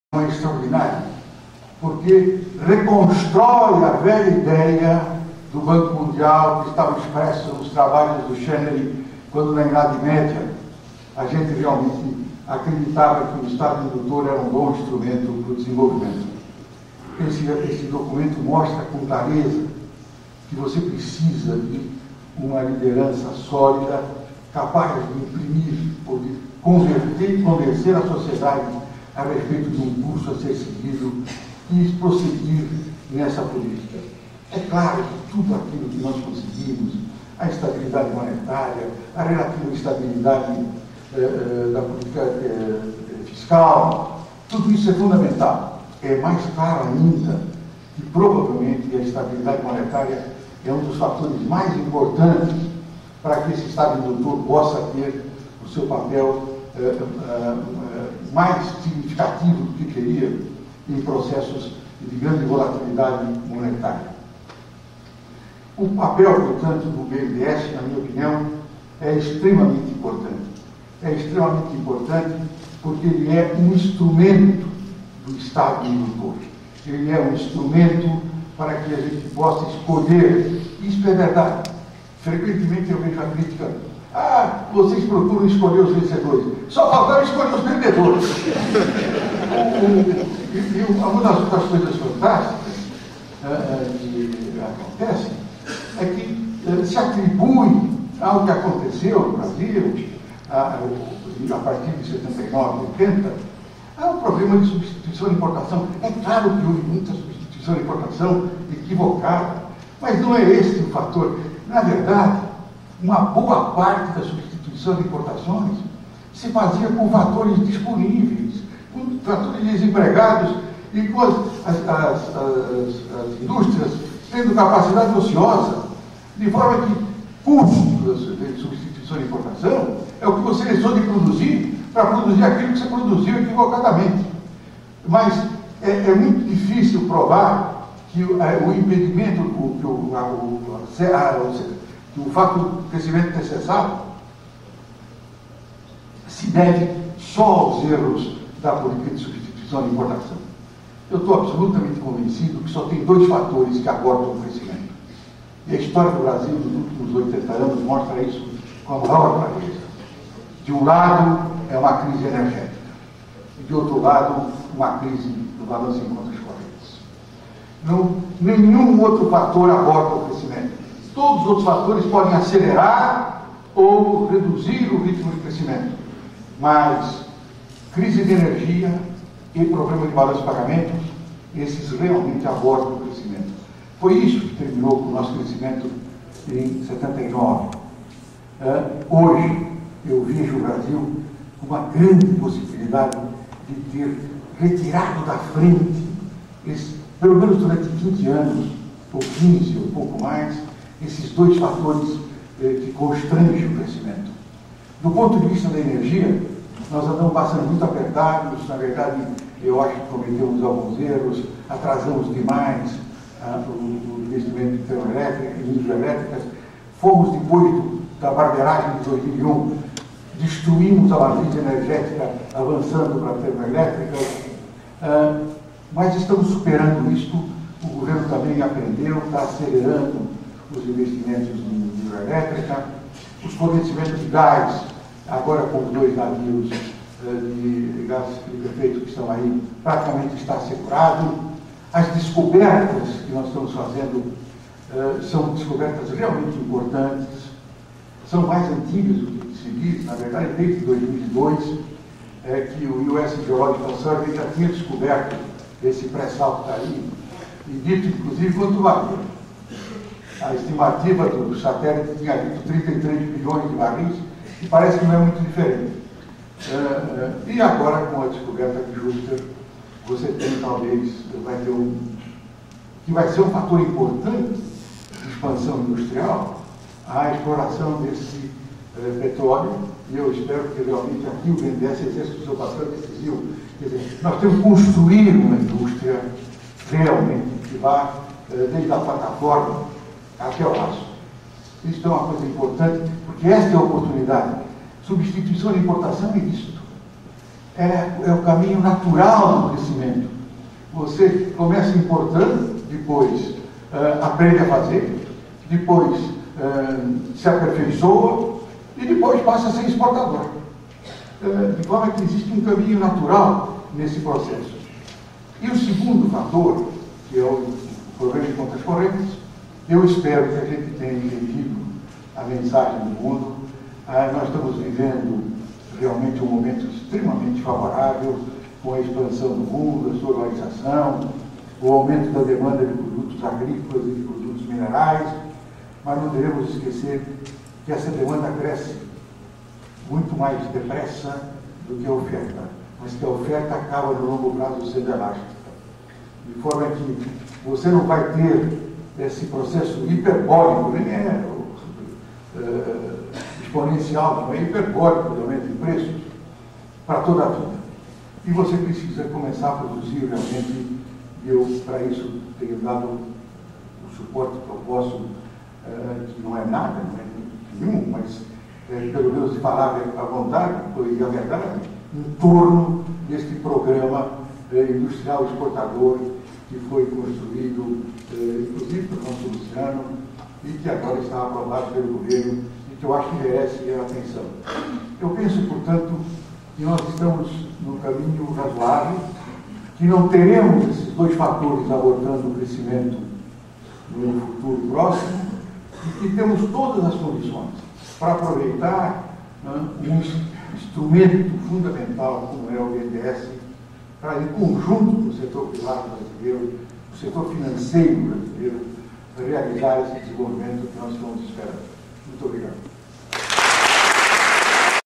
Financimento X Desenvolvimento II
Seminário "Financiamento de Longo Prazo e Bancos de Desenvolvimento - 56º Aniversário BNDES - Delfim Netto II